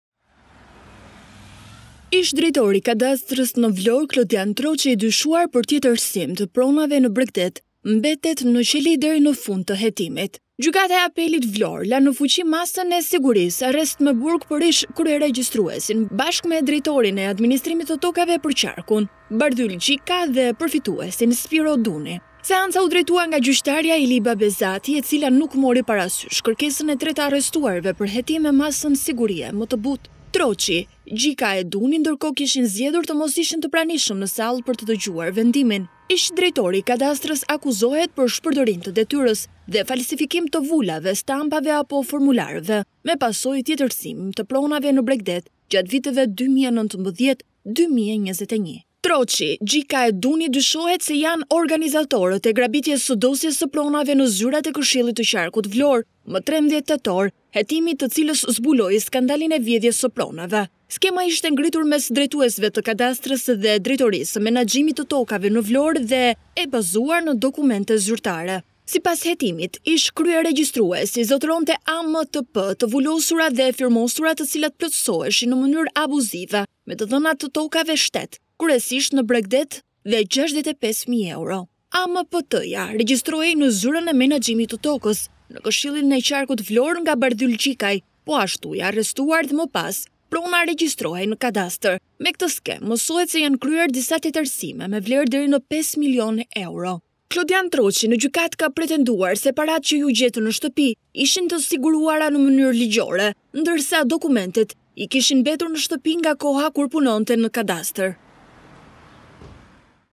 Ish-kreu i Kadastrës Vlorë dhe dy të pandehurit e tjerë nuk bindin Apelin, mbeten në burg
Mbeten në burg ish-kreu i Kadastrës së Vlorës, Klodian Troci dhe dy bashkëtëpandehurit e tjerë.

Gjykata e Apelit në Vlorë ka lënë në fuqi masës e sigurisë "arrest me burg" të vendosur nga shkalla e parë edhe pse Troci së bashku me Bardhyl Gjikaj dhe Spiro Dunin kërkuan një masë më të butë.

 Të pandehurit nuk janë të pranishëm në seancë por u përfaqësuan nga avokatët e tyre mbrojtës.

Troci kërkoi masë më të butë sigurie me pretendimin se shumën prej 60 mijë eurosh e ka përfituar në mënyrë të ligjshme.

Për më tepër ai pretendon se provat që ju gjetën në banesën e tij i kishte marrë pasi la detyrën dhe nuk ishin aty për falsifikim dokumentash. Teksa shprehet se në kujdestari ka edhe një djalë të vogël për të cilin nuk ka kush të kujdeset. Spiro Duni nga ana tjetër kërkoi arrest shtëpie për shkak të problemeve të shëndetit, pasi masa e sigurisë i është komunikuar në spital. Sipas pretendimit të Dunit, ai nuk mund ta përballojë jetën ne qeli për shkak të problemeve shëndetësore.

Bardhyl Gjikaj, 63 vjeç kërkon masë sigurie më të butë për shkak të moshës.

Troci u arrestua me dyshimin se qëndronte në krye të një skemë të tjetërsimit të pronave shtet në bregdetin e Himarës dhe Dhërmiut.

Ish-kryeregjistruesi bashkëpunonte për vjedhjen e pronave me kreun e menaxhimit të tokës në Vlorë.